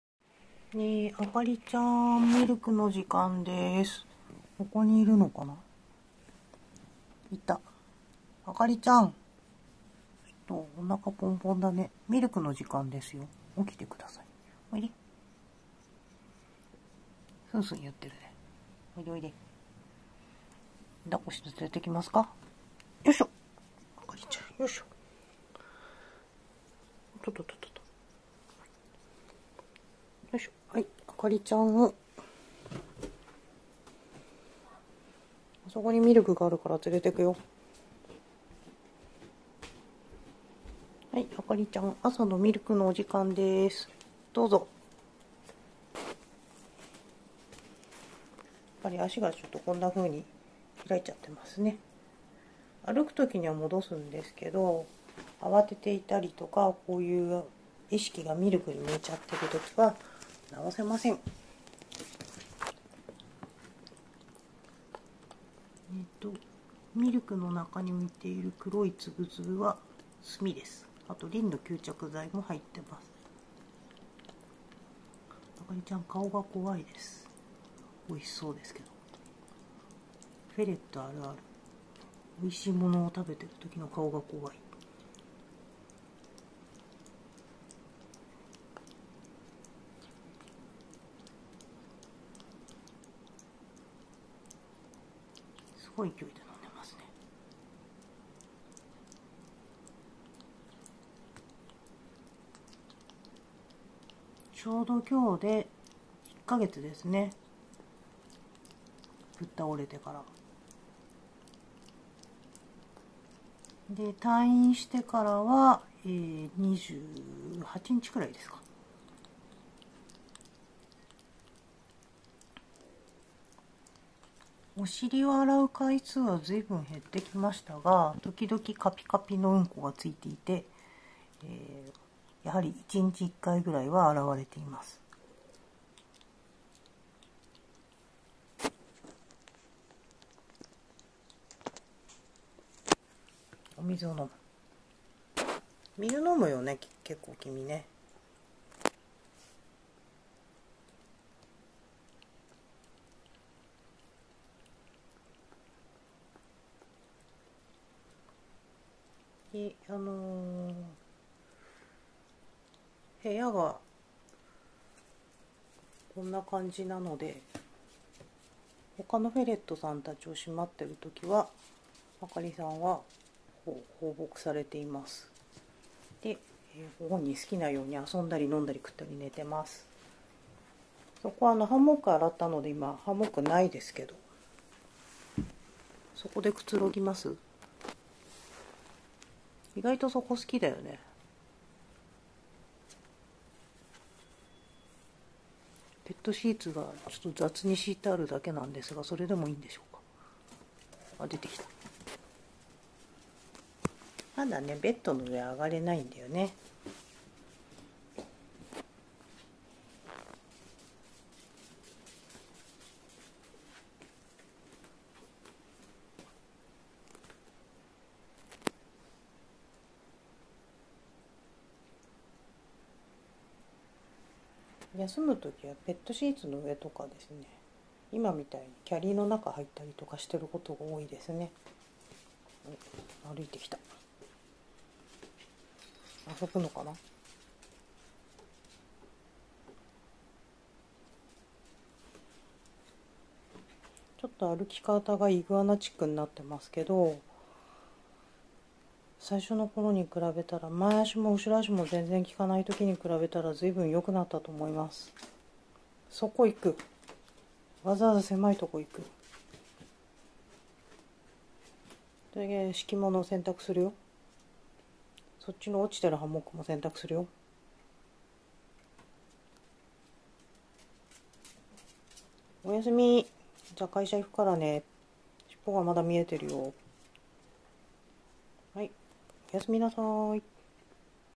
低血糖発作後に神経症状の出たアンゴラフェレットさん 1ヶ月たちました
ちょうど1ヶ月。同じような動画ですが、日常の生活がほぼできるように。
普通に生活できるって。やっぱり幸せなことです。